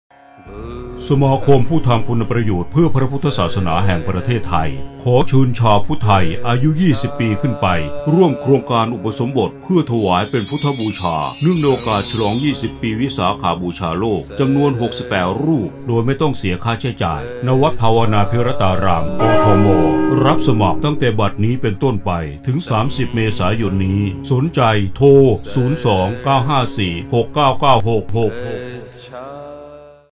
สปอตทีวี โครงการอุปสมบทเพื่อถวายเป็นพุทธบูชาเนื่องในเทศกาลฉลอง 20 ปี วิสาขบูชาโลก 2562
สมาคมผู้ทำคุณประโยชน์เพื่อพระพุทธศาสนาแห่งประเทศไทย
ขอเชิญชาวพุทธไทยอายุ 20 ปี ขึ้นไป ร่วมโครงการอุปสมบท
ถวายเป็นพุทธบูชาเนื่องในเทศกาลฉลอง20ปีวิสาขบูชาโลก 2562
จำนวน 68 รูป โดยไม่ต้องเสียค่าใช้จ่าย ณ วัดภาวนาภิรตาราม กทม.
รับสมัครตั้งแต่บัดนี้-30 เมษายน 2562